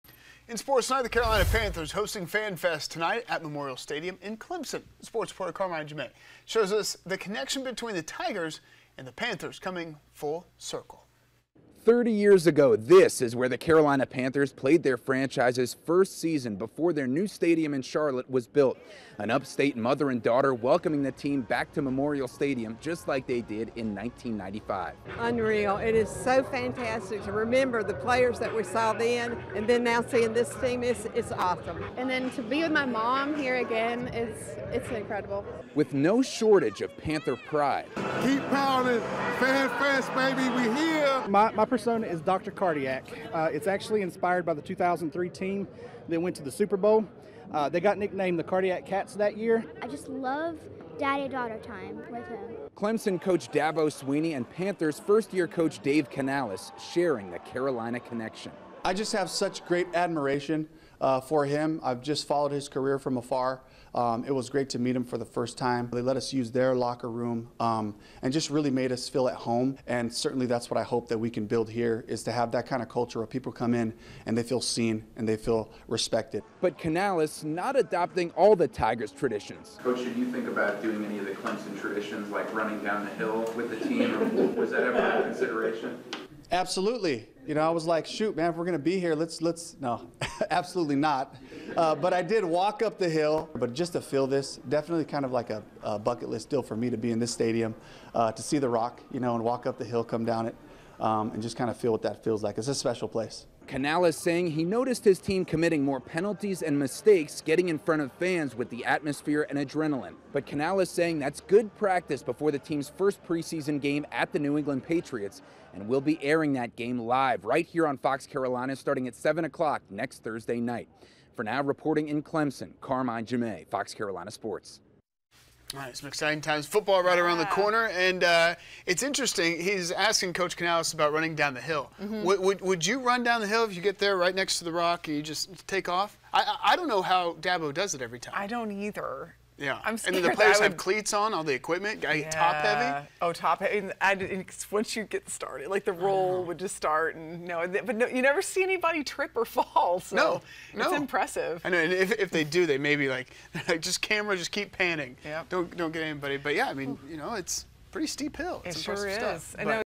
Clemson welcomes Panthers back home for Fan Fest
The Carolina Panthers played their first season at Memorial Stadium in 1995, and Thursday they received the same warm welcome at Fan Fest for the 30th season.